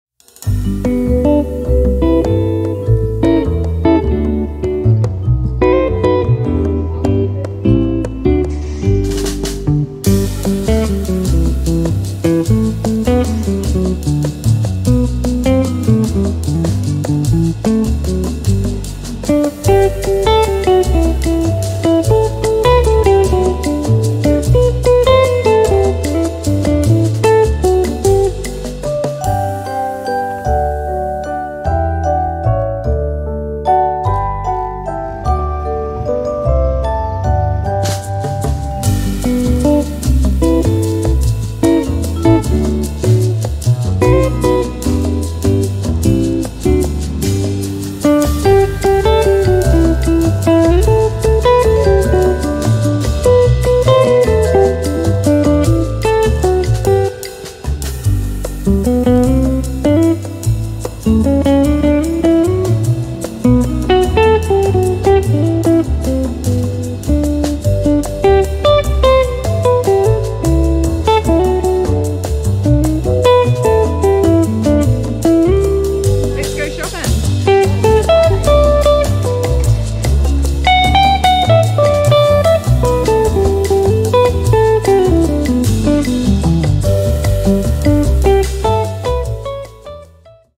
Christmas Shopping Spree Te Puke 2022
Mike and Beth from Aim Spray Painters Christmas Shopping like a pro in Te Puke, Dec 2022.
@She_Is_Me_NZ (Insta)